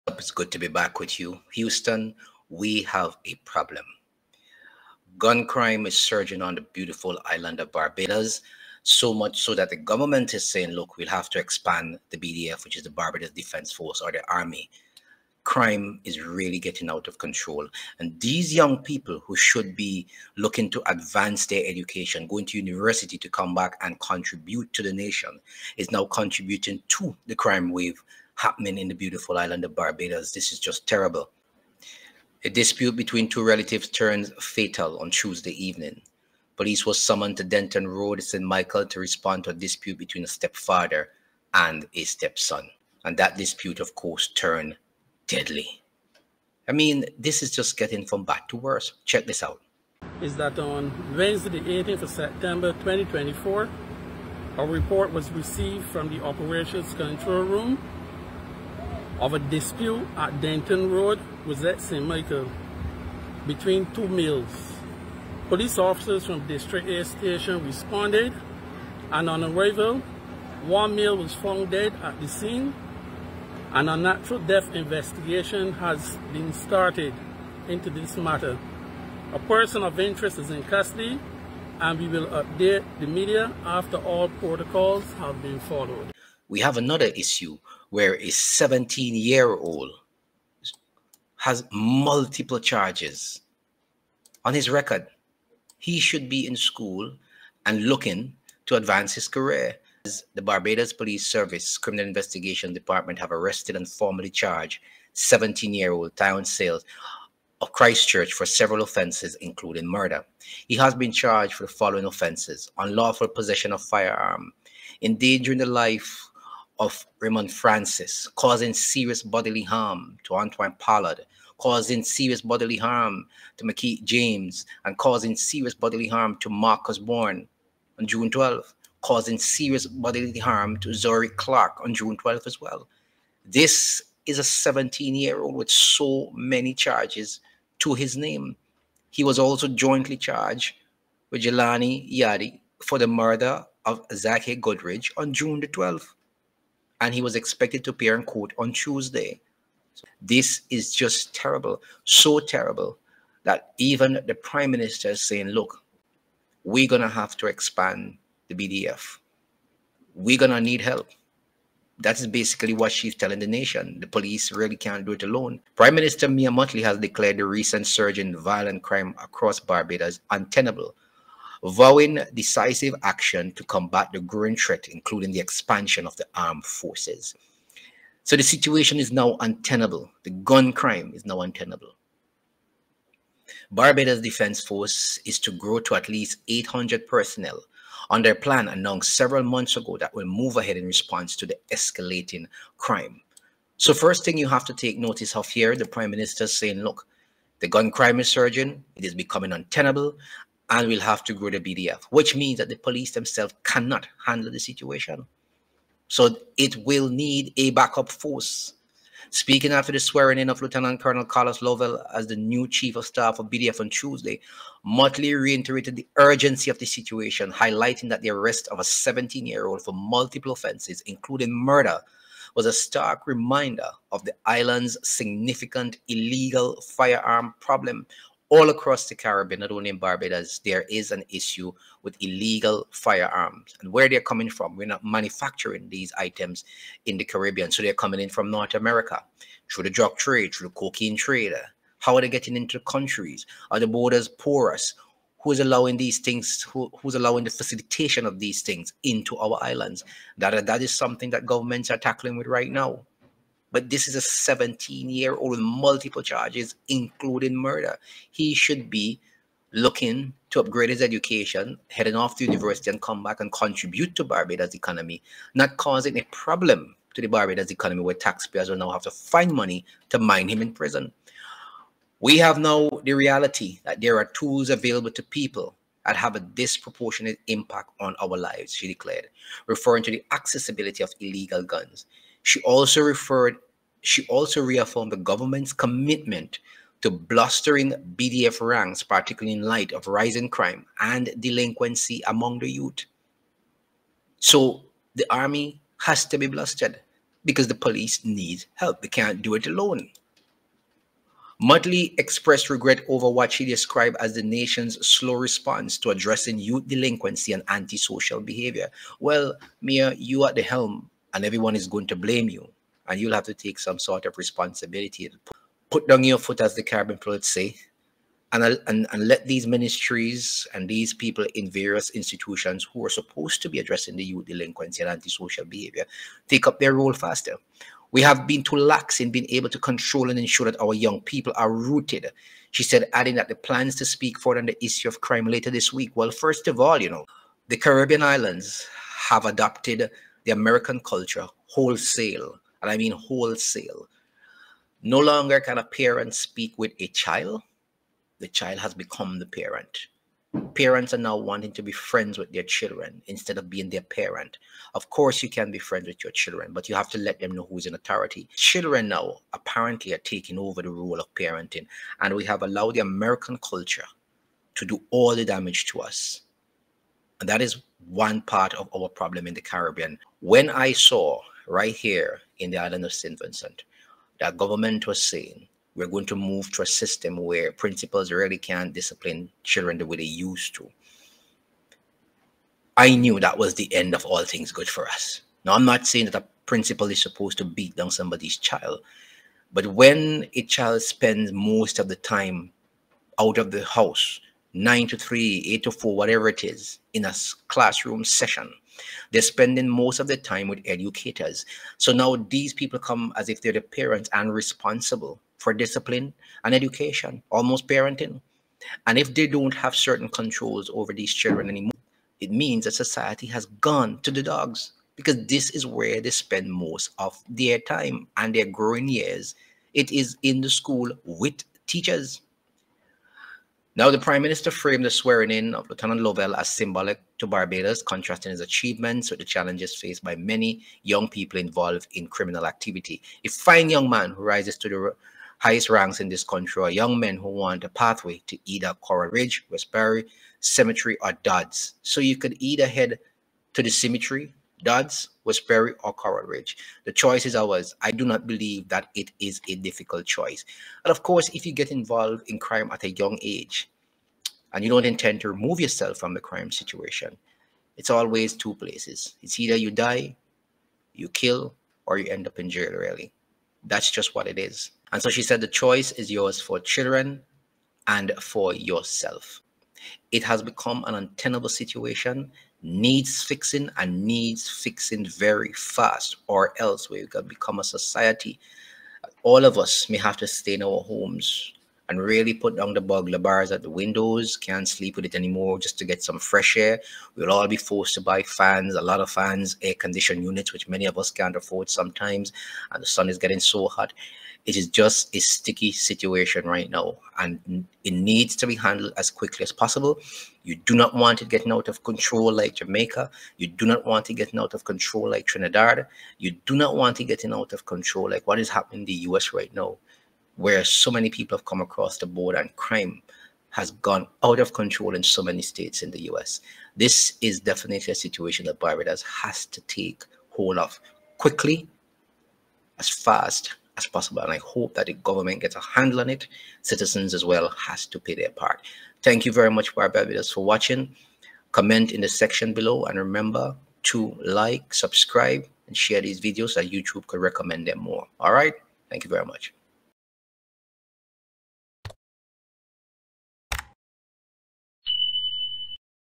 Barbados In Crisis! Gun Crimes Surge..Army To Recruit More People.
The Barbados Defence Force (BDF) is to grow to at least 800 personnel under a plan announced several months ago that will move ahead in response to escalating crime.